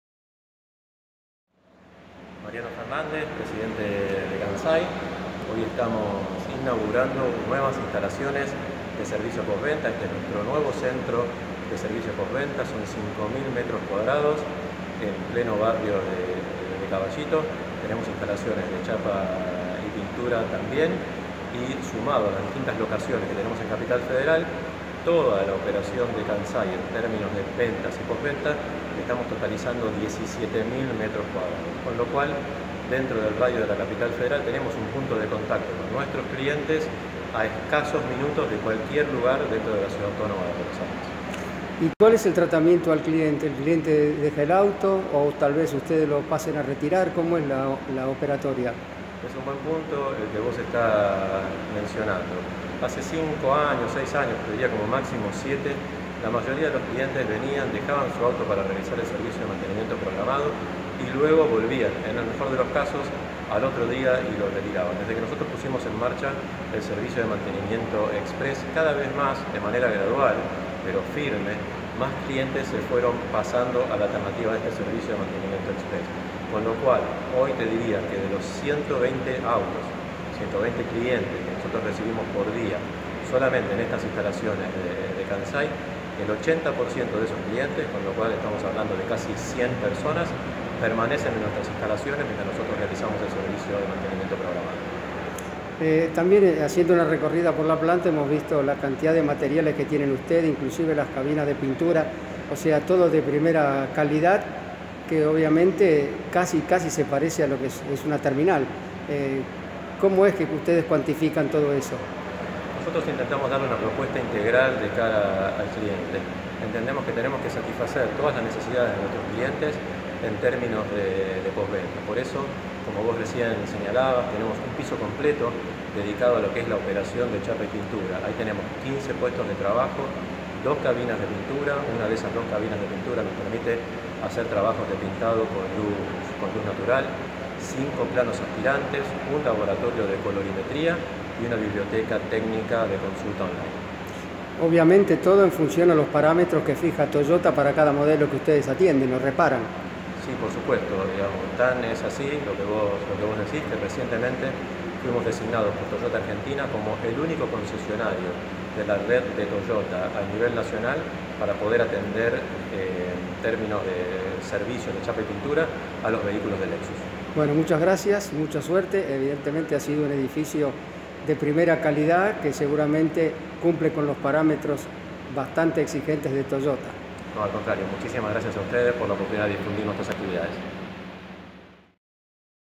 Dr. Mariano Fernández, Presidente de Kansai. Nuevo Centro de Servicio de Posventa Toyota.
Kansai inauguró su nuevo servicio de posventa. Más de 5.000 m2 distribuidos en 4 plantas, dedicados a brindar los más altos estándares de calidad, atención y servicio para todos los usuarios de la marca Toyota. Las nuevas instalaciones están ubicadas en Av. Tte. Gral. Donato Álvarez 1350, centro geográfico de la CABA.